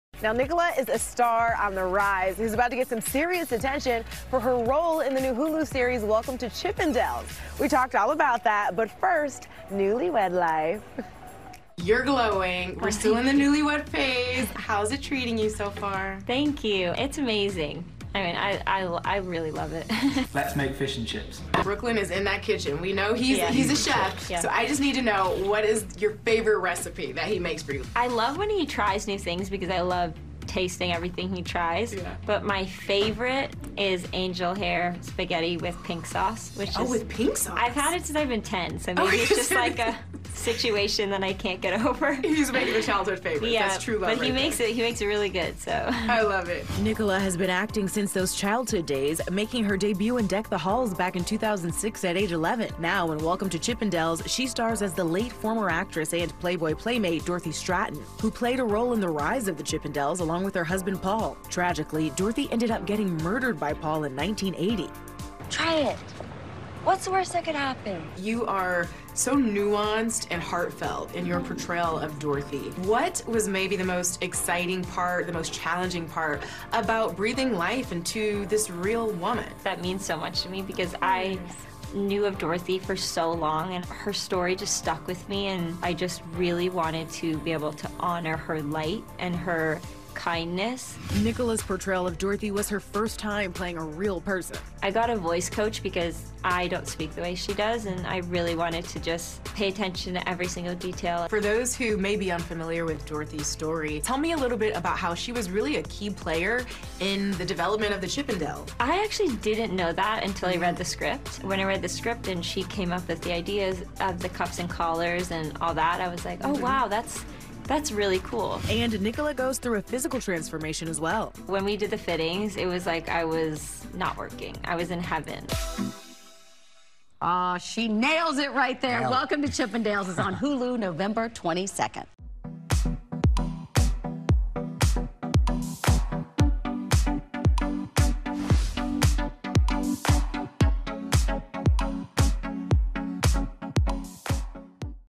Nicola Peltz Beckham Gushes About Being Married To Brooklyn Beckham: ‘I Love It’
Nicola Peltz Beckham is telling Access Hollywood’s Zuri Hall how newlywed life is treating her! “It’s amazing, I really love it,” she said. The actress married photographer Brooklyn Beckham in a lavish wedding in April. Her husband is also an aspiring chef, and she is telling Access what her favorite dish he makes is. Since getting married, Nicola has been busy! She is starring as Dorothy Stratten in the mini-series “Welcome to Chippendales” and is opening up about transforming into the late model. “Welcome to Chippendales” premieres on Hulu Nov. 22.